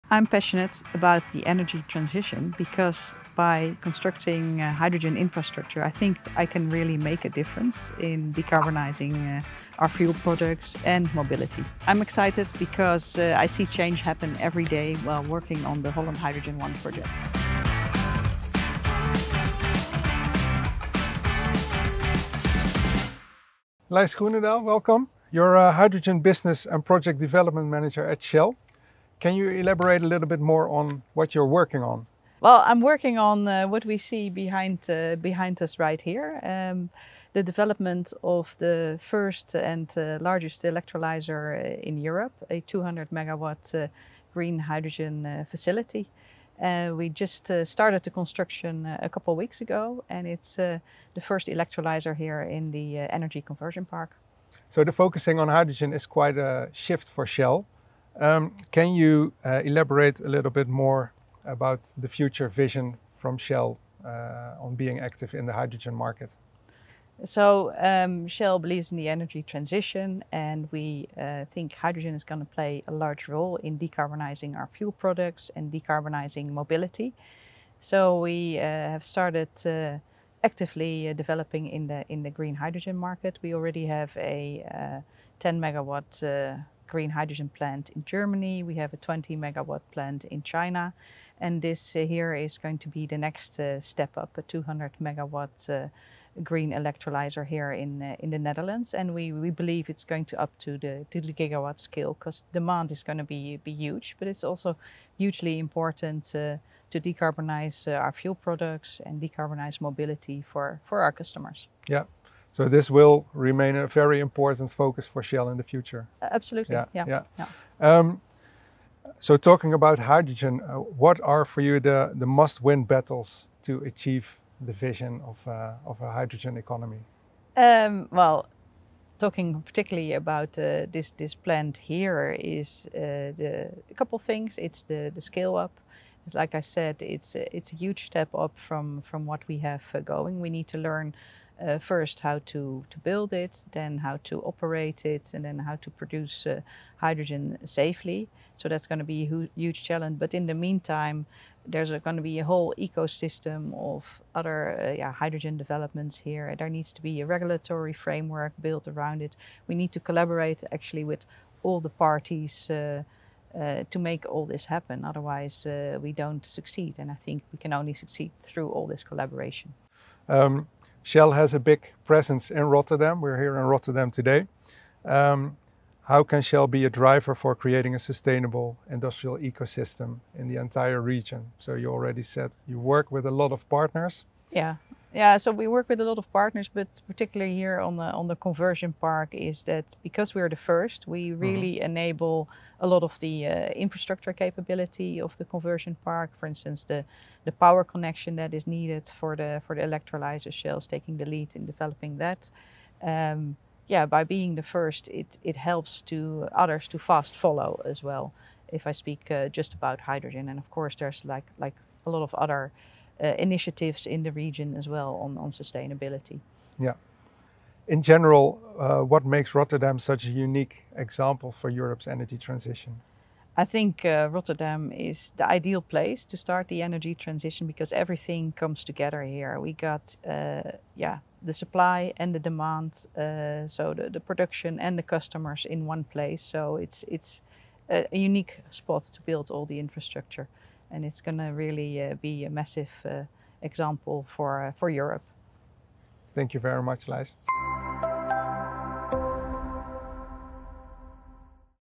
Shell’s electrolyser in Rotterdam: A building block in the hydrogen economy
Lijs Groenendaal, Shell’s Hydrogen Business and Project Development manager, explains Europe’s largest electrolyser: Shell’s Holland Hydrogen 1 project will form a key building block in the emerging hydrogen mobility market in The Netherlands and the broader region.

HH1 is leading the way in green hydrogen technology in Europe and will eventually will produce an average of 68 tons of green hydrogen per day.